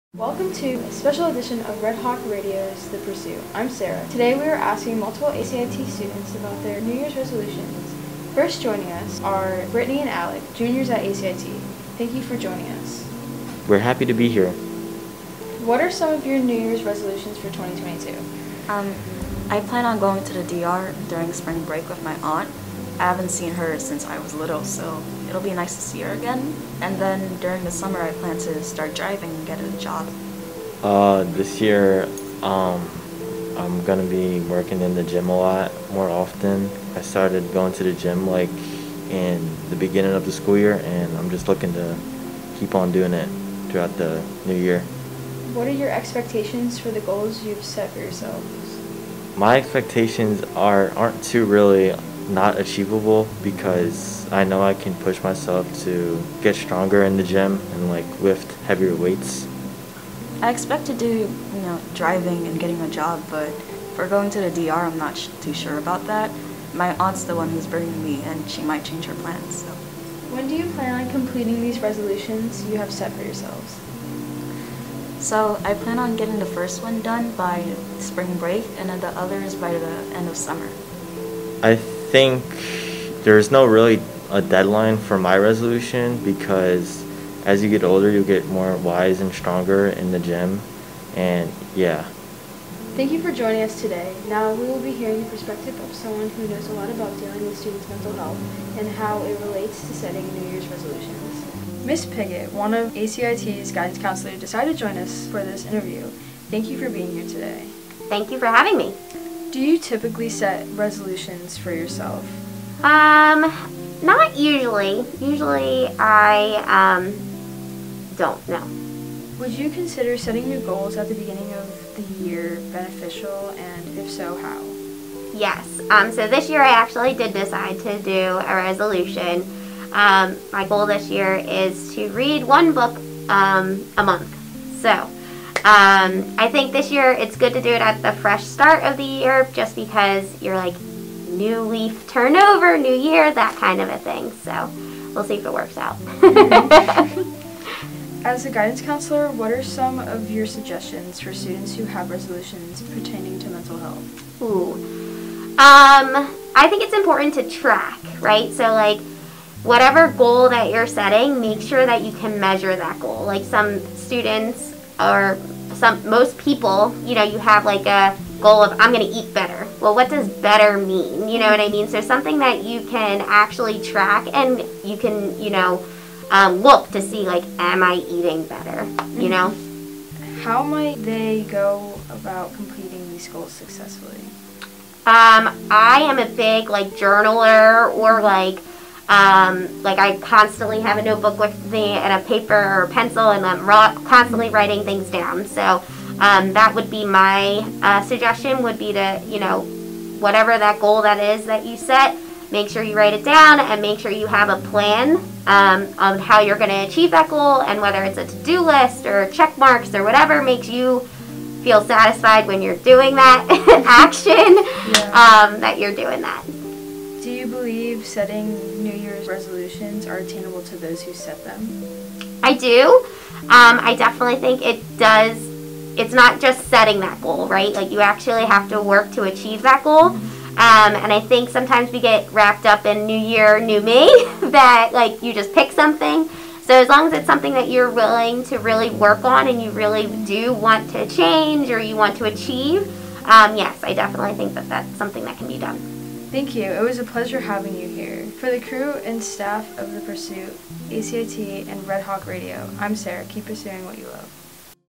Red Hawk Radio Podcast - New Year's Resolutions Around ACIT
Red Hawk Radio Reporter, Sarah Nicholas, sat with several classmates to discuss their thoughts about making New Year's Resolutions and whether they made a few themselves.

Produced & Edited by: Sarah Nicholas
News Directors & Executive Producers: Bayly DiPilato & Brian Lacivita